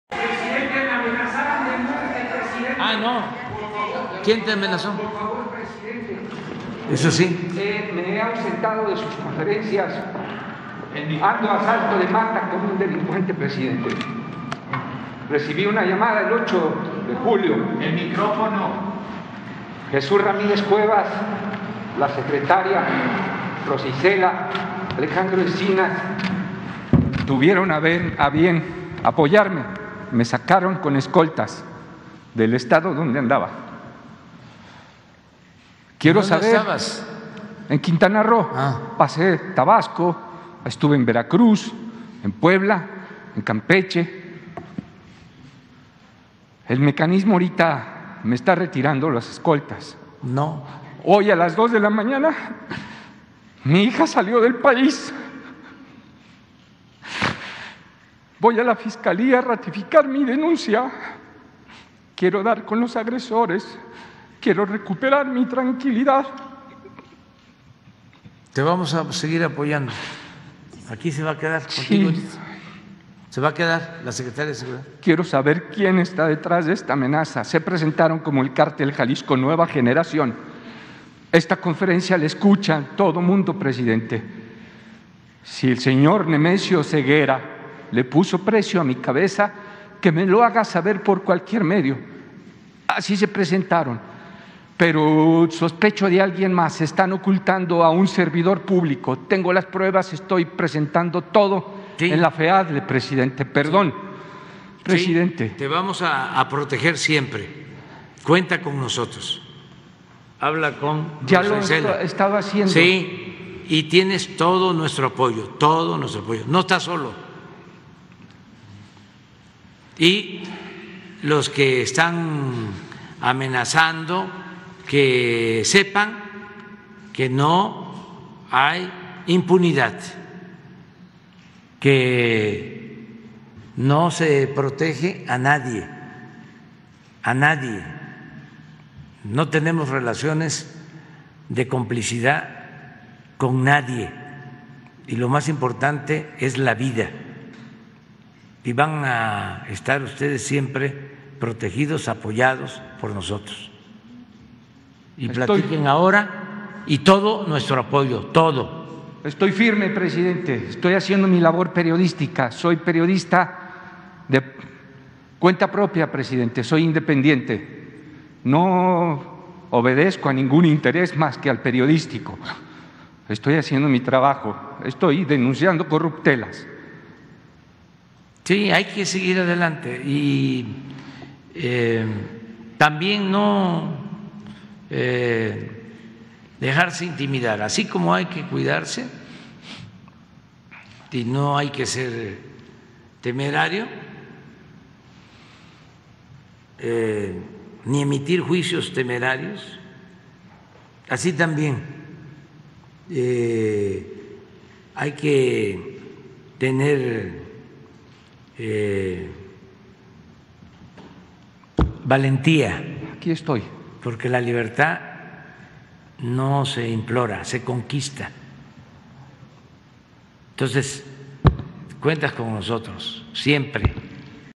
"No estás solo" dice AMLO a reportero Rodolfo Montes, quien denunció amenazas de un grupo delictivo
Al concluir la mañanera, el presidente se regresa para escuchar al reportero Rodolfo Montes, quien denunció que recibió amenazas de un grupo delictivo: "No estás solo, los que están amenazando, que sepan que no hay impunidad"